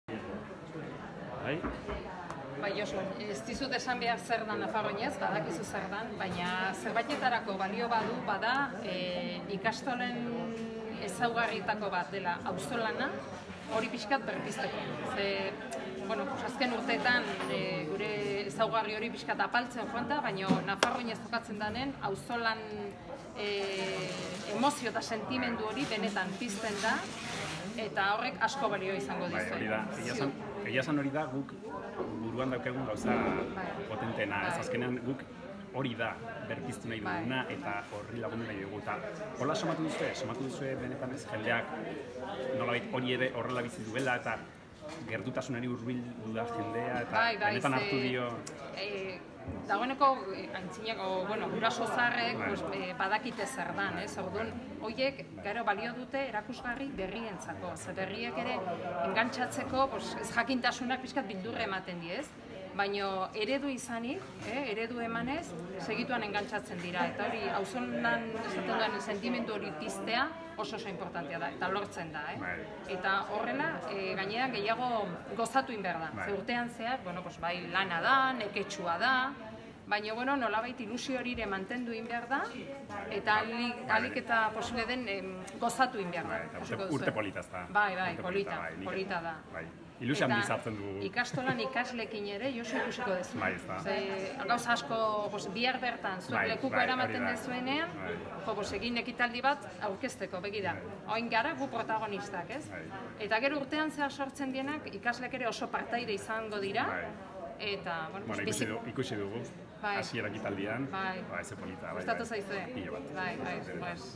Nafarroa Oinez
Tuterako Argia ikastolako Anika Lujan eta Lizarra Ikastolako buru Iosu Sueskun jarri ditugu aurrez aurre, Nafarroa OInezen lekukoa hurrengo urtean Lizarrak hartuko duela baliatuta.